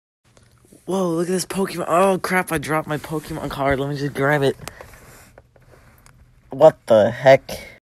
RANDOM STRAY DOG FOUND IN MY HOUSE?!
NEVER SEEN THIS DOG IN MY LIFE WTF